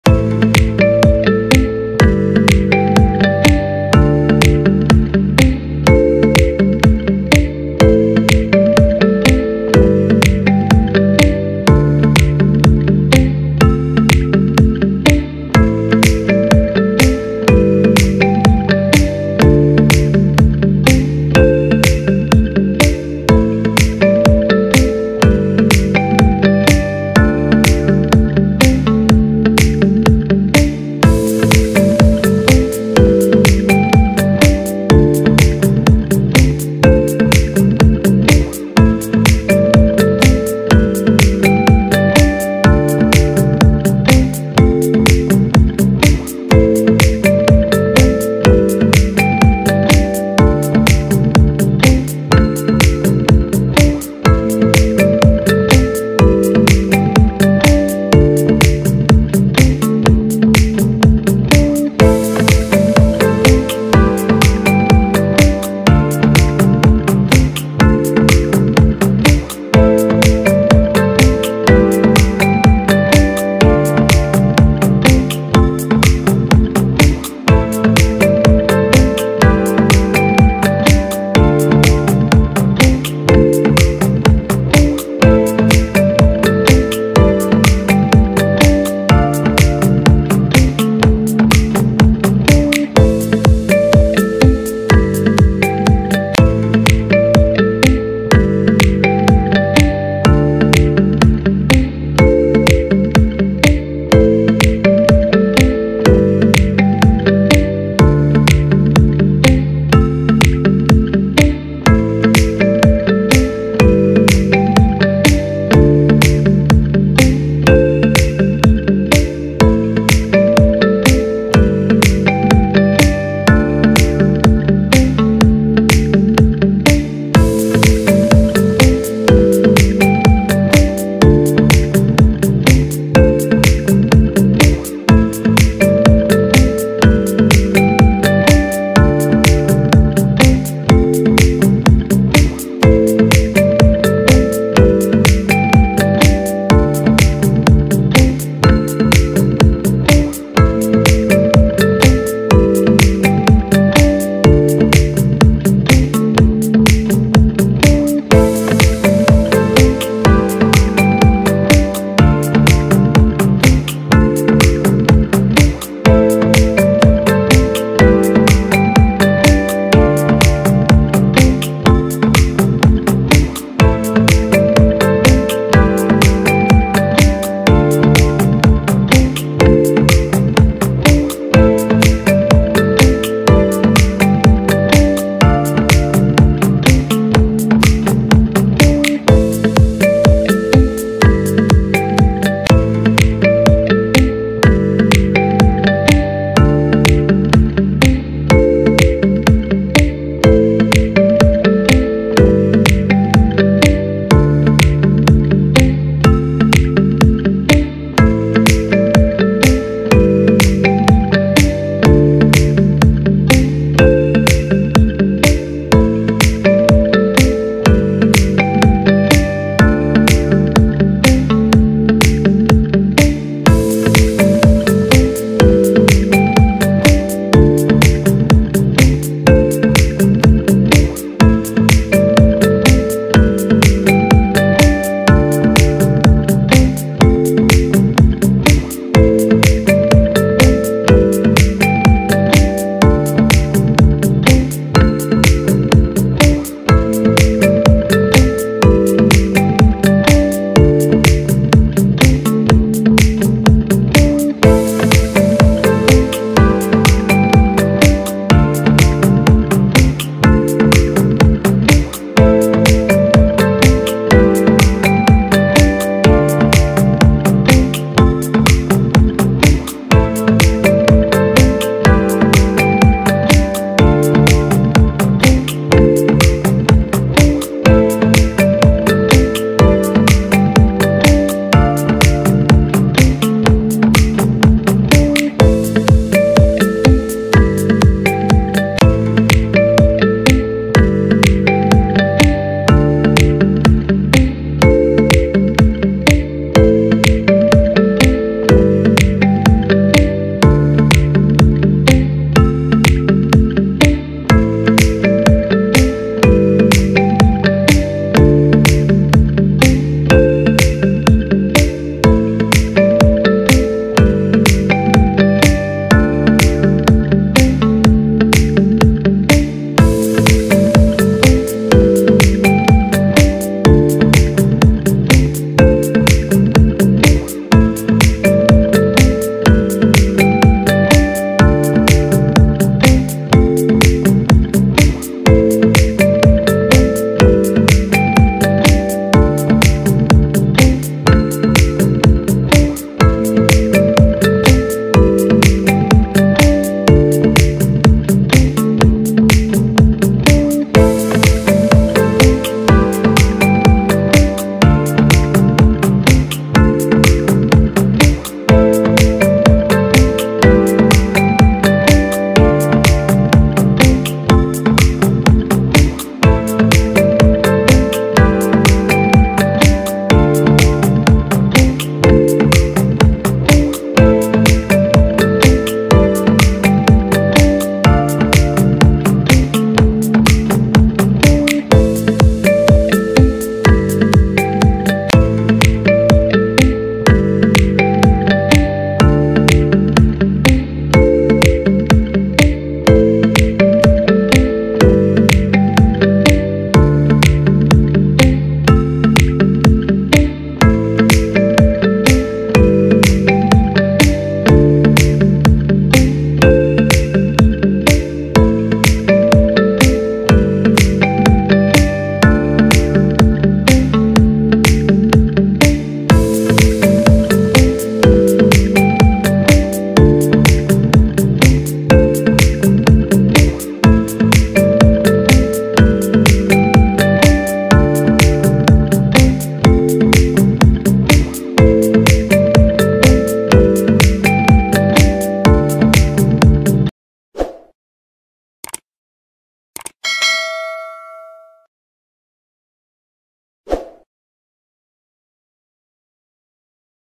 जोंक पालन का रहस्य: कैसे चीन उपभोग के लिए अरबों जौ की खेती करते है? Leech Farming Secrets in Chaina
Discover the fascinating world of leech farming in China! This in-depth video explores the innovative techniques and sustainable practices used to cultivate billions of leeches. Learn how these incredible creatures are farmed and processed for consumption, and uncover the cultural and economic significance behind this unique industry. Whether you're interested in aquaculture, traditional medicine, or simply curious about leeches, this video has something for everyone.